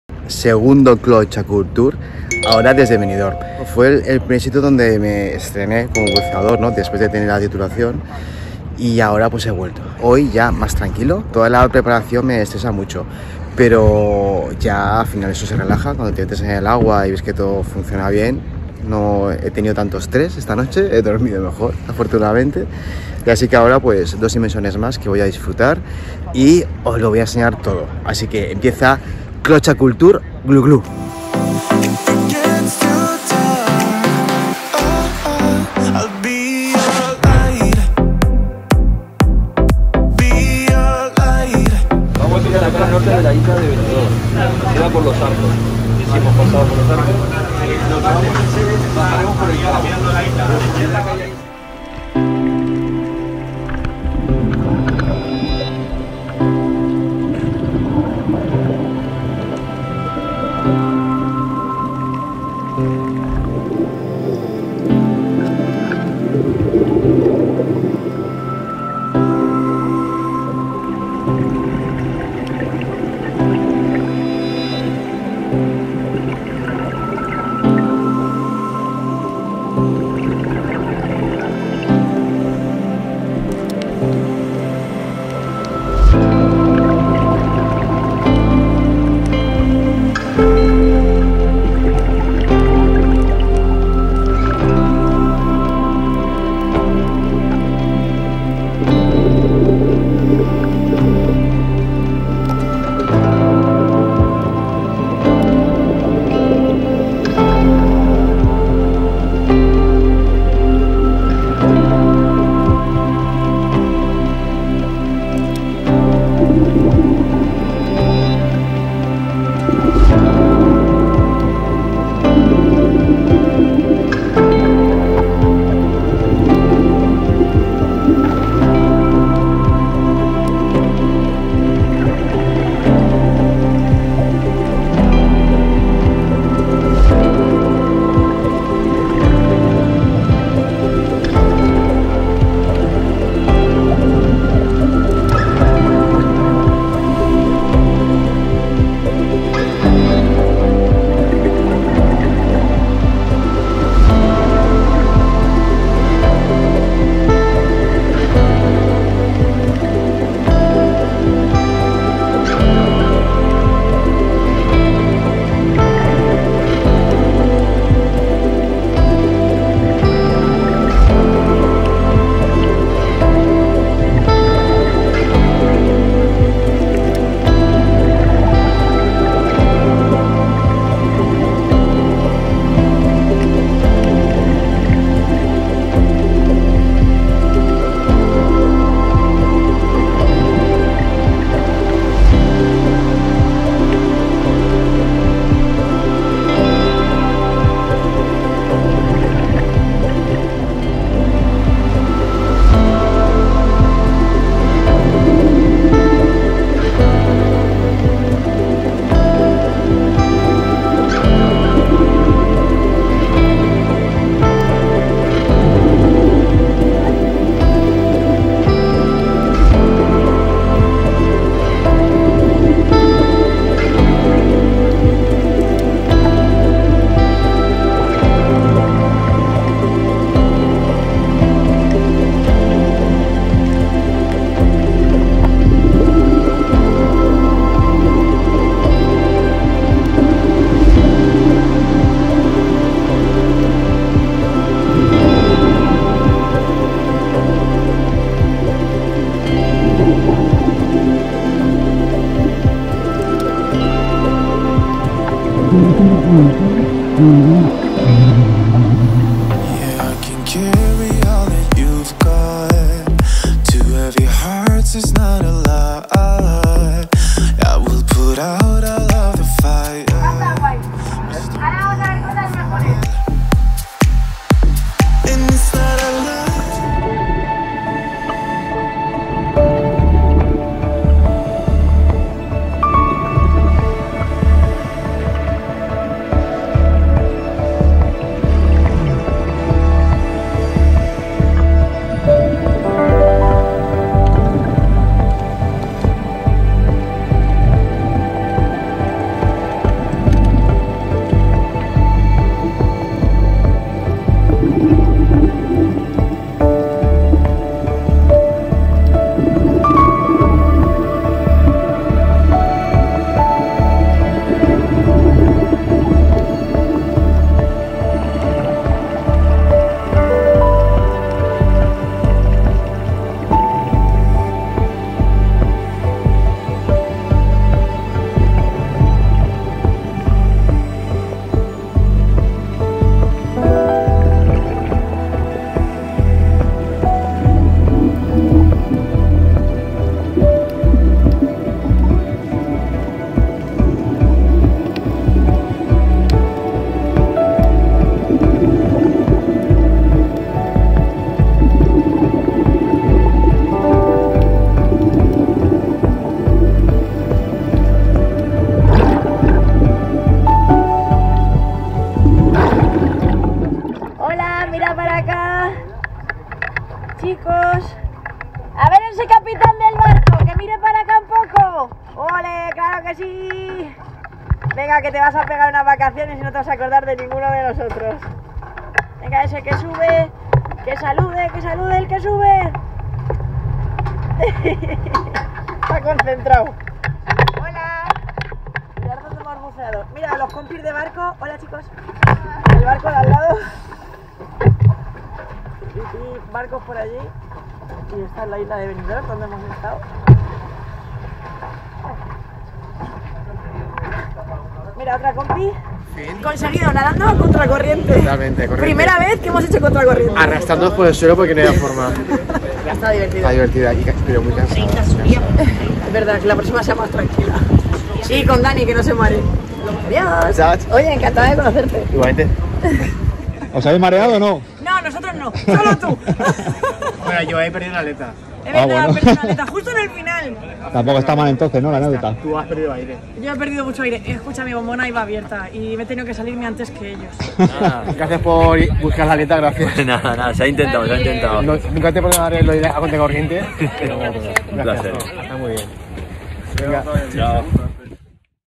CCTGluGlu | Benidorm (2021)
Centro buceo Diving Stones, en Benidorm.
Dos inmersiones:
  1.L'illa de Benidorm, norte: 61 min, 26 ºC, 22 m máx.
  2.L'illa de Benidorm, oeste: 51 min, 27 ºC, 19 m máx.

-flickr: CLOTXA Produccions